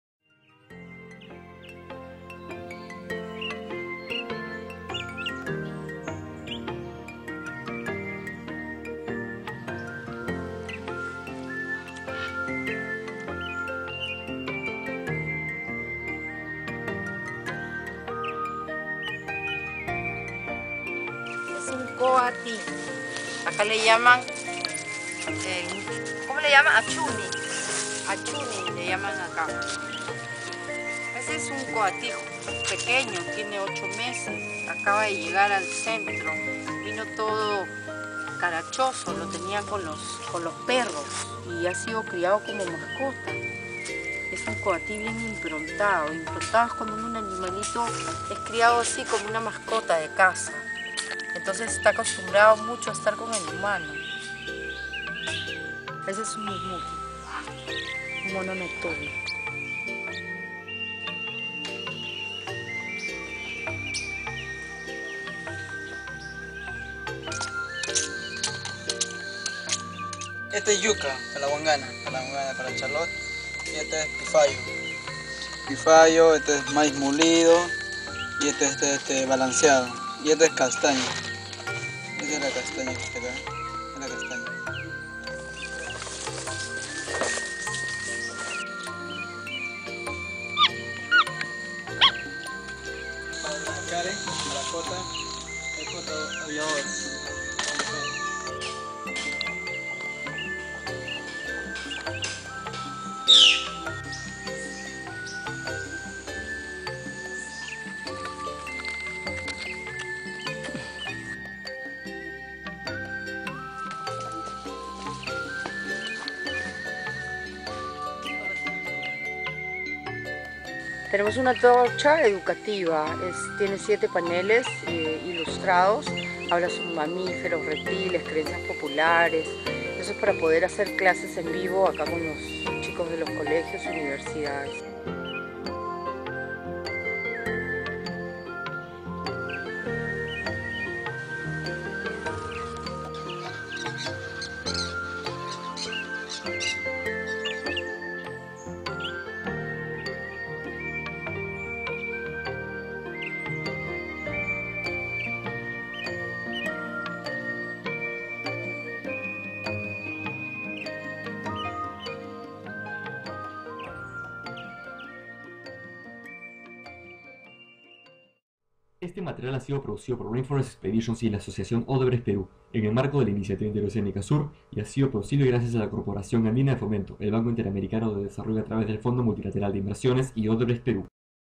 Amazon Shelter - The Stay & Activities
Amazon Shelter is located 45 minutes by road away from Puerto Maldonado. This is an animal rescue center where volunteers can work with the animals.